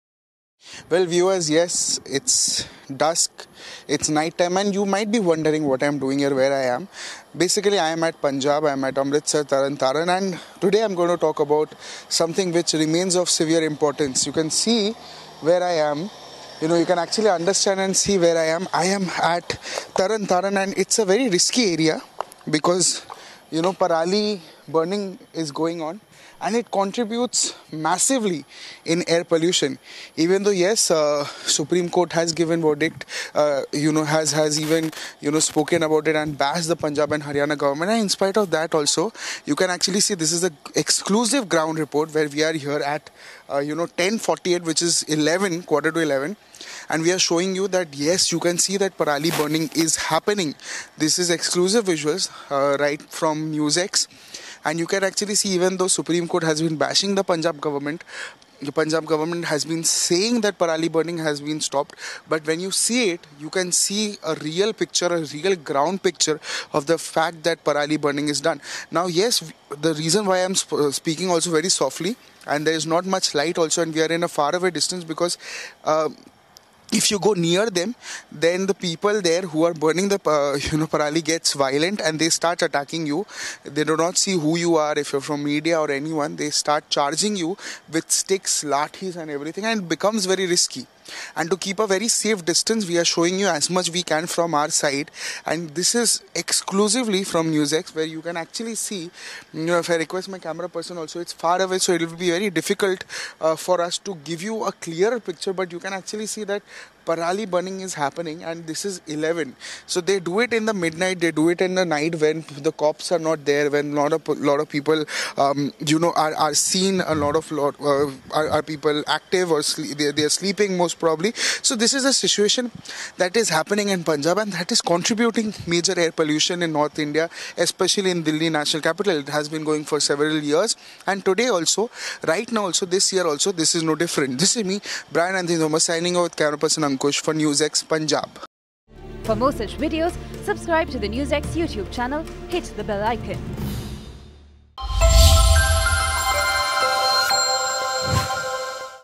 Overnight Stubble Burning In Amritsar, Punjab | Exclusive Parali Report | NewsX
In Amritsar, Punjab, overnight stubble burning has intensified, causing severe air quality deterioration and health hazards for residents. This exclusive NewsX report investigates the alarming increase in parali burning, highlighting its impact on the environment and public health. As the local community grapples with the consequences, there is a pressing need for immediate action and sustainable solutions to address the stubble burning crisis. The report sheds light on the ongoing struggle against pollution and its effect on the region's well-being.